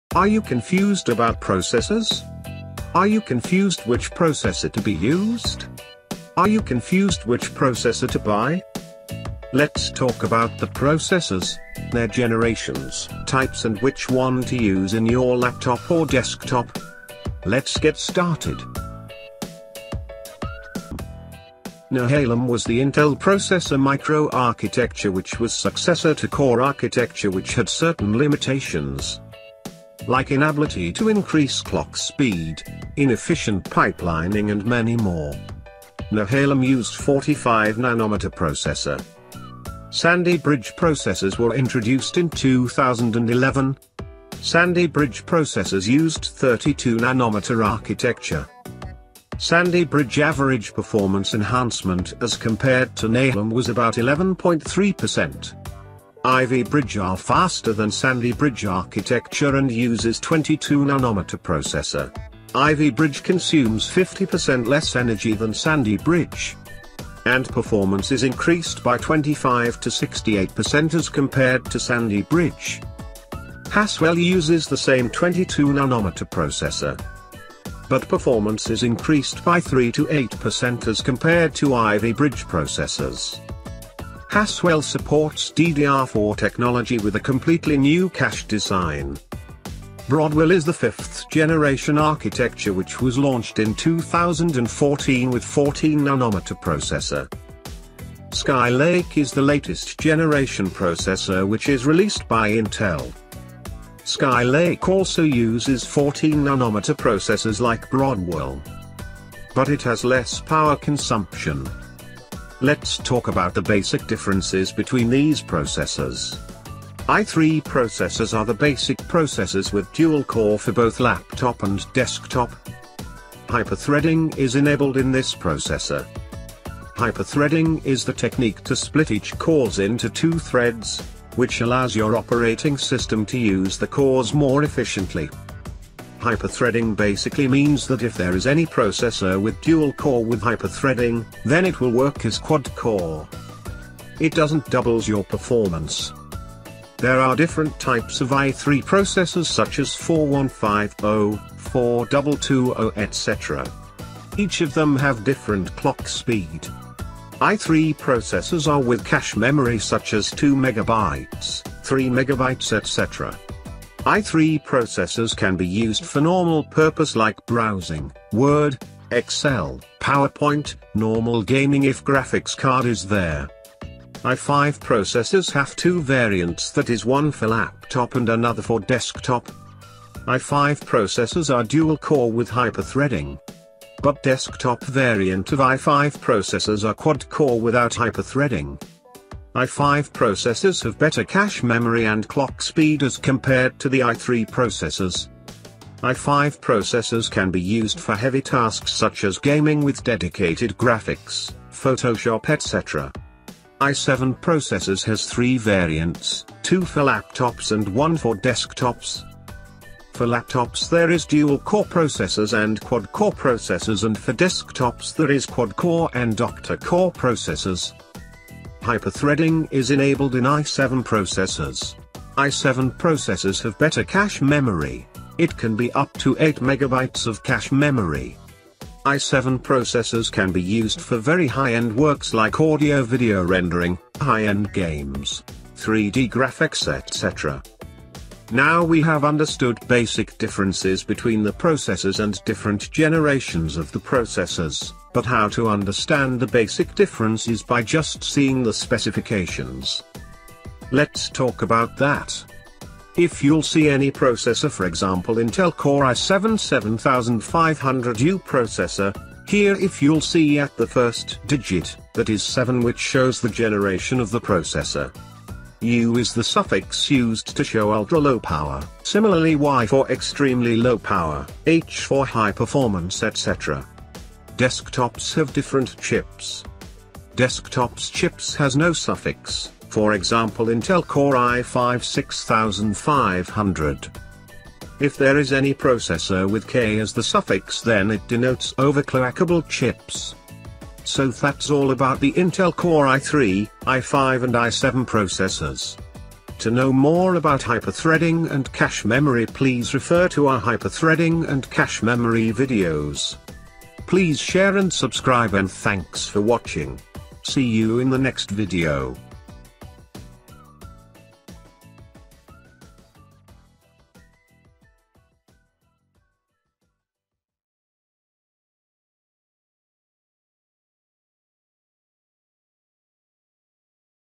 i3 vs i5 vs i7 : ALL YOU NEED TO KNOW ABOUT || BUYING GUIDE
first of all, many people think that core i3,i5 and i7 are the processor generations. But, you should know , that these are models or brands of processors from intel .

The concept of generations mainly came after the release of core i series .The difference in processor micro architecture is the main difference in processor generation.

As the processor generation changes , we are moving to a smaller processor , means the CPU is physically smaller ,which also means it uses less power, but maintains the same clock frequency .

Let's talk about different processor generations and how to differentiate in between them